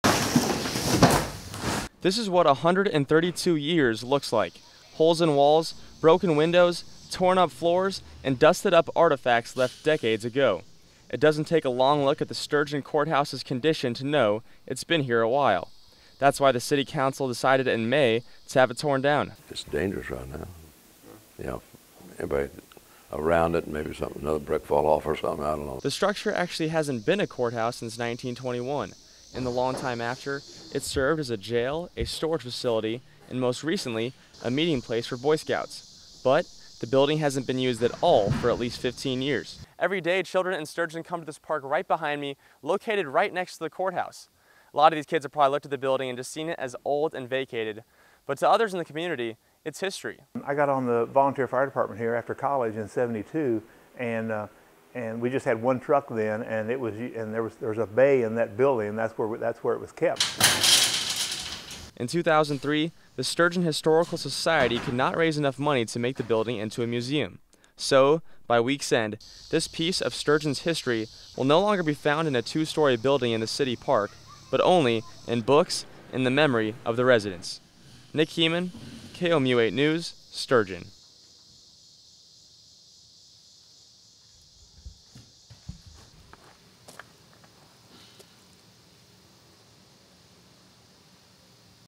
Excavation crew set to demolish historic Sturgeon courthouse
Story done for my Broadcast 2 class at the University of Missouri.

Anchor Intro:

For one Mid-Missouri town, today marks the end of a significant piece of its history. Around noon, an excavation crew will begin demolition on a Sturgeon Court of Appeals that has stood since 1882. KOMU 8's Nick Hehemann tells us what the courthouse means to some of the town's residents.

Tag: Mayor Kelly says there are no finalized plans yet, but the city has talked about building either a gazebo or another basketball court in the building's place.